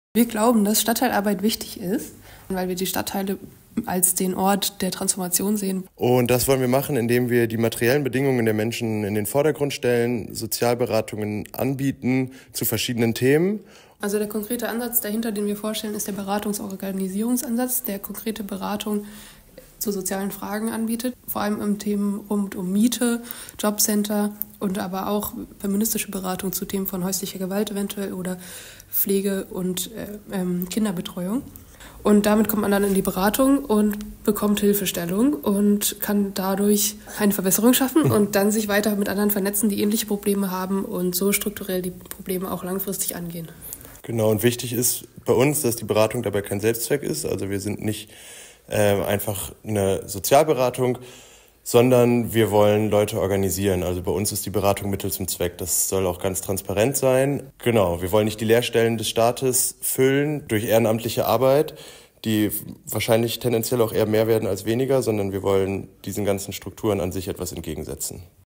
Stadtteilgewerkschaft
Was machen unsere Studierenden eigentlich in ihrem Studium?
Zum Beispiel die Demokratisierung von Stadtteilen planen!
Im Bachelor "Ökonomie - Nachhaltigkeit - Transformation" haben Studierende ein Konzept entwickelt, wie sich Menschen eines Stadtteils gegenseitig helfen und solidarisch organisieren lassen.